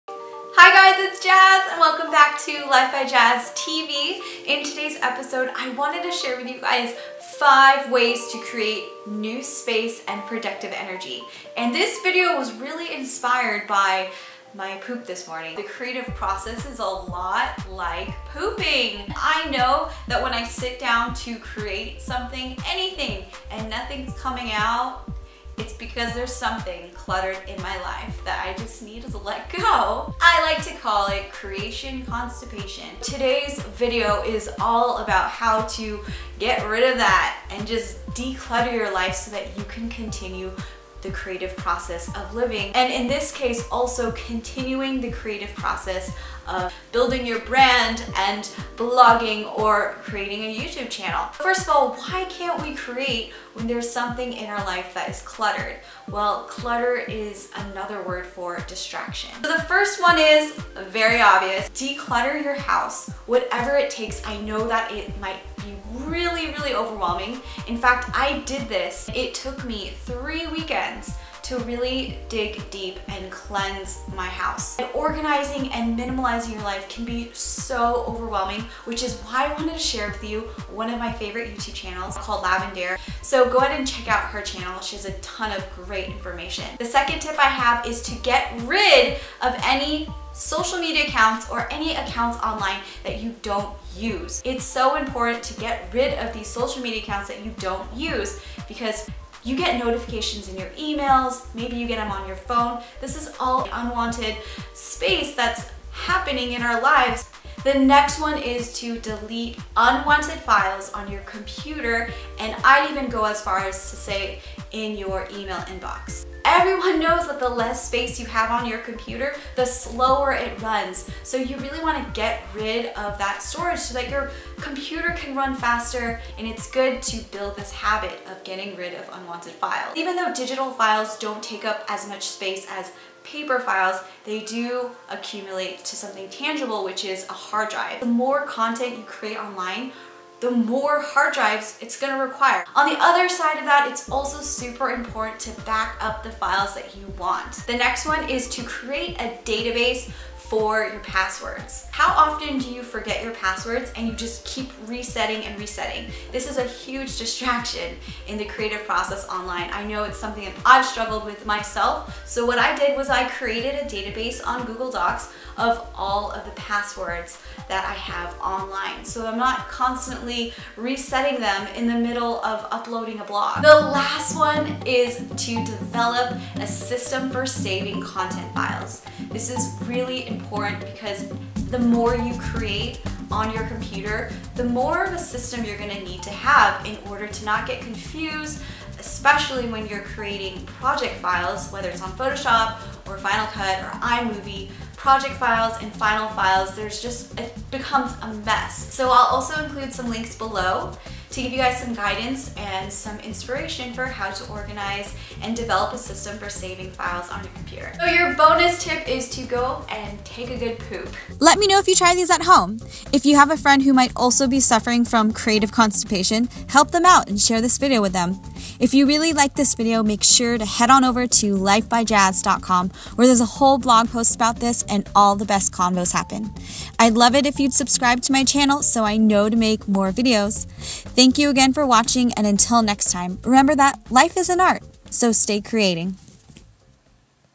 5 Ways to Create New Space & Productive Energy for Creating
If you've ever found yourself stuck in the creating process - you must watch this video!

Decluttering your life does wonders for your creative game! In this video, I share 5 ways to create new space & productive energy.

5 Ways to Create New Space & Productive Energy to Create
1. Declutter your home & workspace
2. Get rid of any unused social media accounts
3. Delete unused unnecessary files from your computer
4. Create a password database
5. Develop a system for saving files
Bonus: Go take a poop!

Filmed & Edited by LifeByJazzTV